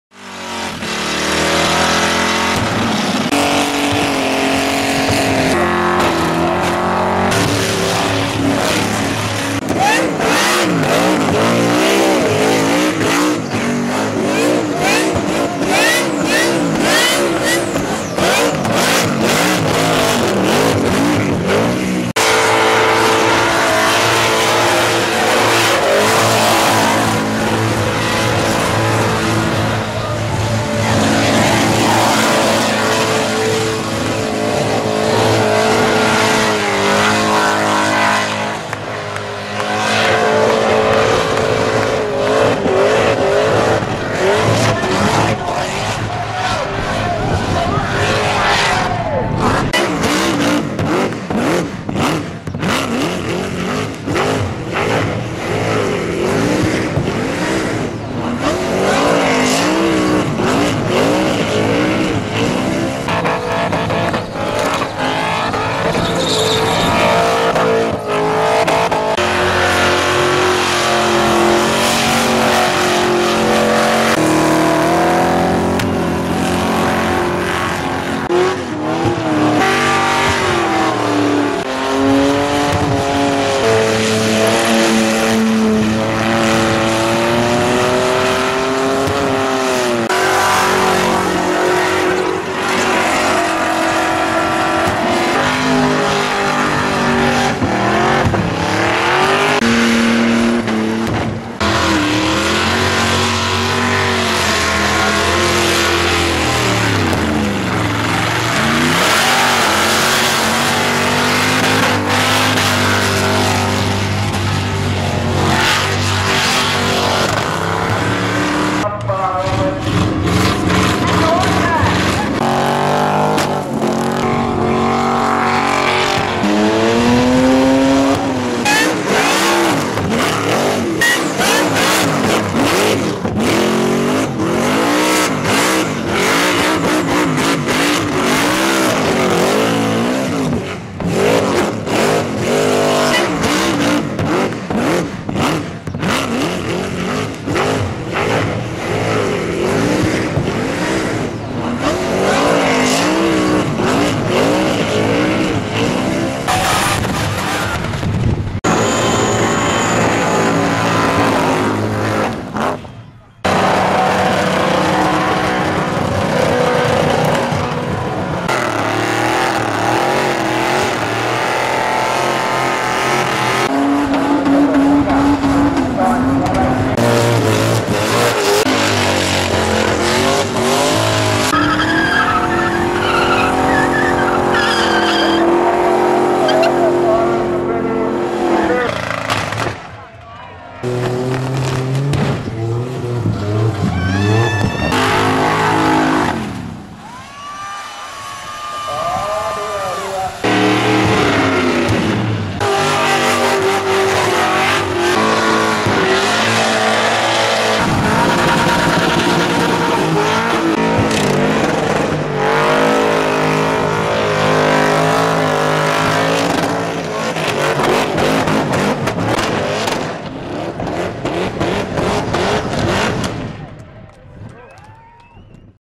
One Night Stand Bunbury 2012 Flaming Highlights
Flames, tyre pops and carnage at One Night Stand Bunbury 2012.